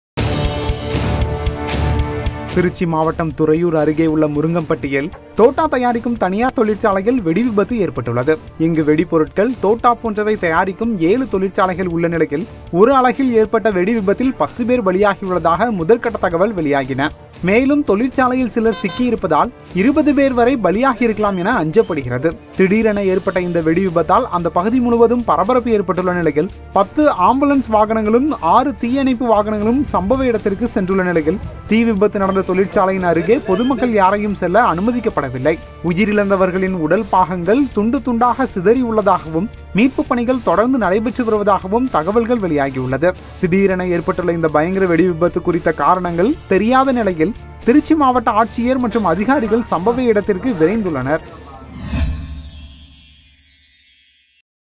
Trichy Fire: Ten Die in Blast at Fireworks Factory
16 people were killed after a blast ripped through a fireworks factory in Thuraiyur town of Tamil Nadu’s Tiruchirapalli district on Thursday morning.